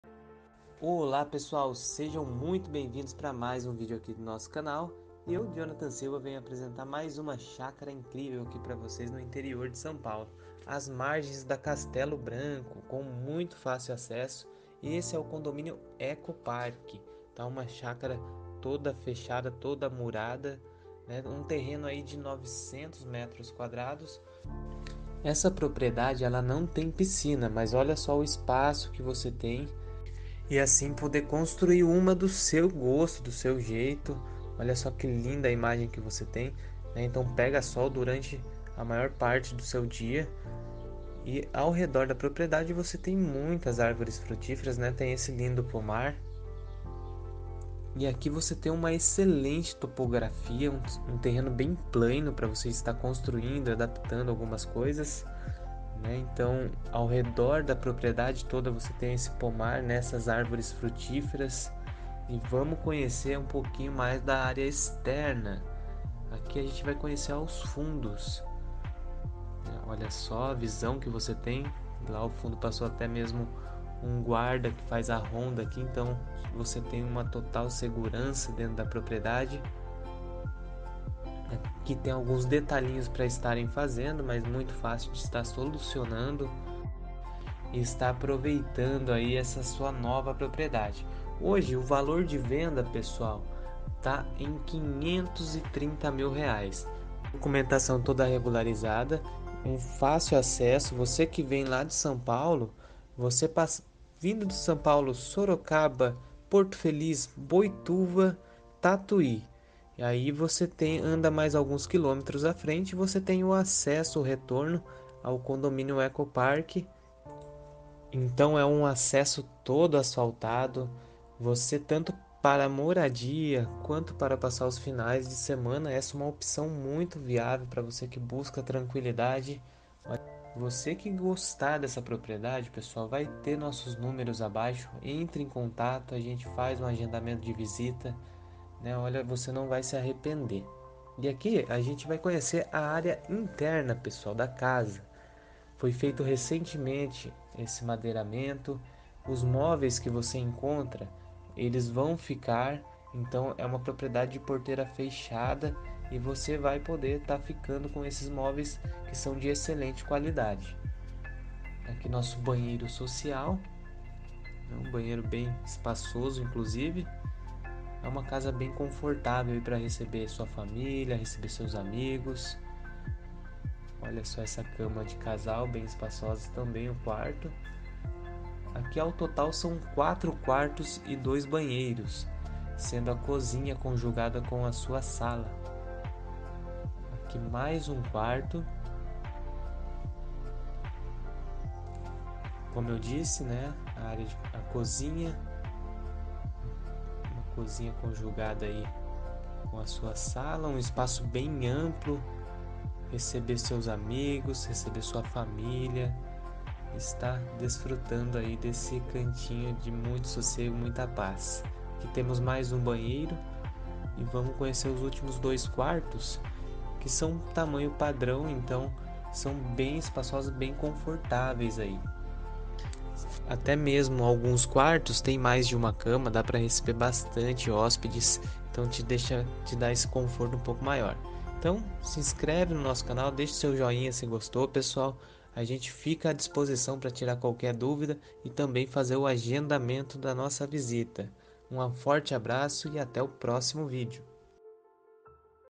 920m² - Chácara à venda em Tatuí/SP - CONDOMÍNIO ECOPARK - Rodovia Castelo Branco-R$ 530.000.00
Imóvel localizado as margens da Rodovia Castelo Branco, na cidade de Tatuí SP, uma aconchegante propriedade, para passar seus dias de descanso.

Condomínio: Ecopark
Terreno: 920 m²
Imóvel contem:

- 4 Quartos
- Sala
- Cozinha
- Lavanderia
- 2 banheiros
- Pomar
- Lindo gramado, podendo plantar ou até mesmo ampliar a construção.

Valor de investimento:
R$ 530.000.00